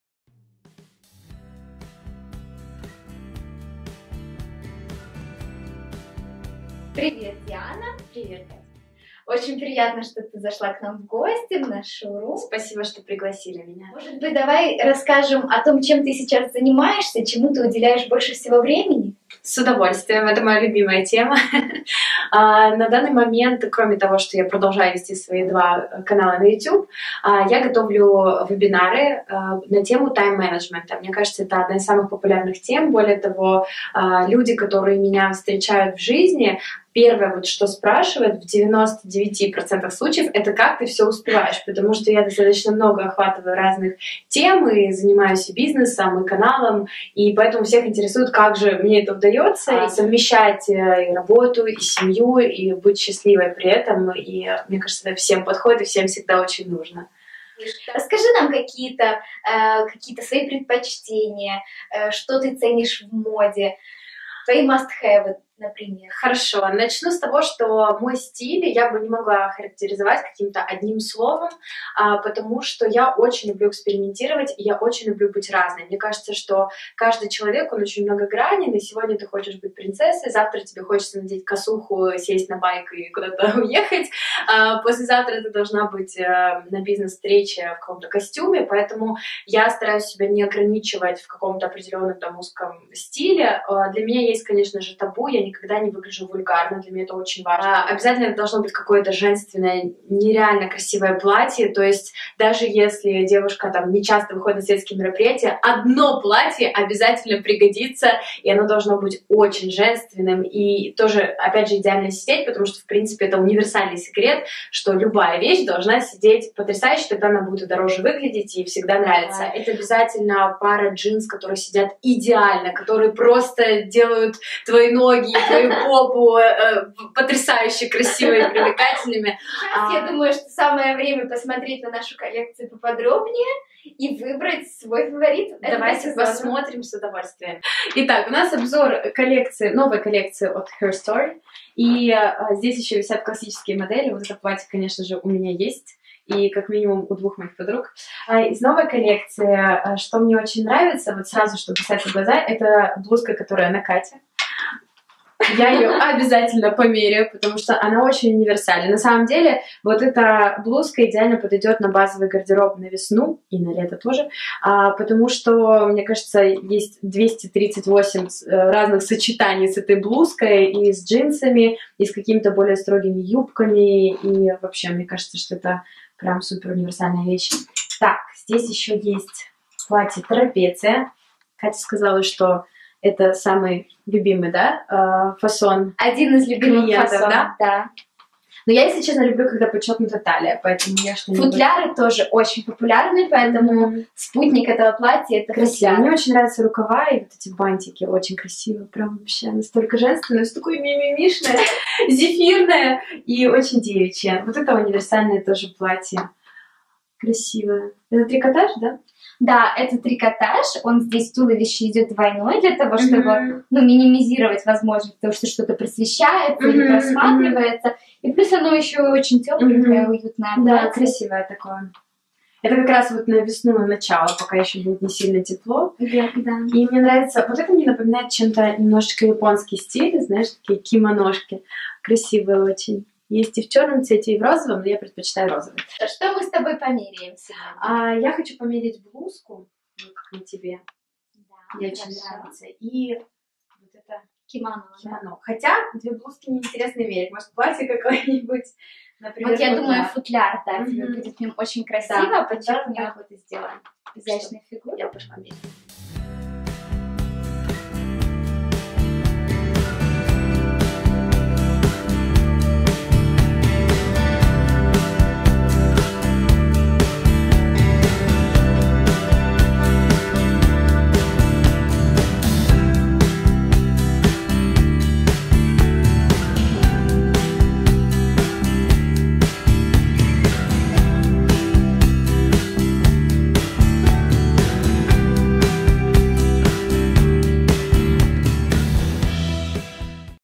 Диана Глостер в гостях у Herstory: cекреты стиля, вещи must-have и любимые платья SS2018
Блоггер, ведущая, дизайнер и просто удивительная девушка Диана Глостер рассказала о том, как ей удается все успевать, оставаться верной своему стилю и о том, какие вещи просто обязательны в гардеробе каждой девушки!

Также Диана выбрала несколько фаворитов в весенней коллекции от Herstory и примерила их в нашем уютном шоуруме.